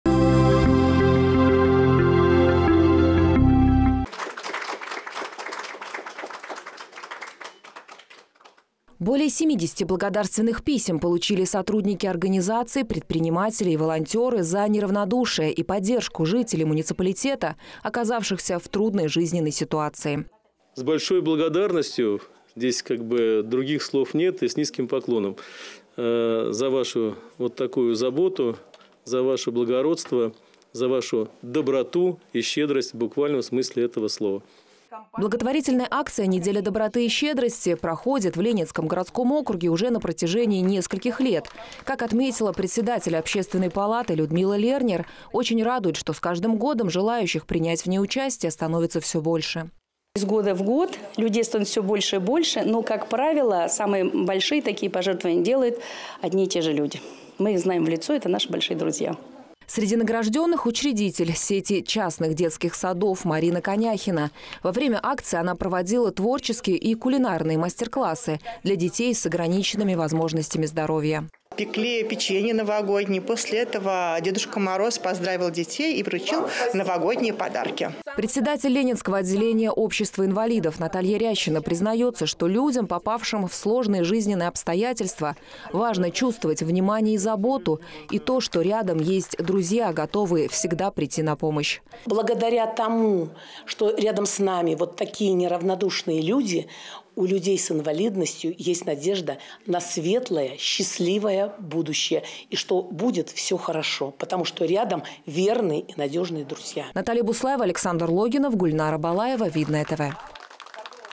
ДОБРЫЕ И ЩЕДРЫЕ
В администрации муниципалитета подвели итоги «недели доброты и щедрости». Члены общественной палаты округа наградили участников акции благодарственными письмами. Подробности в нашем следующем сюжете.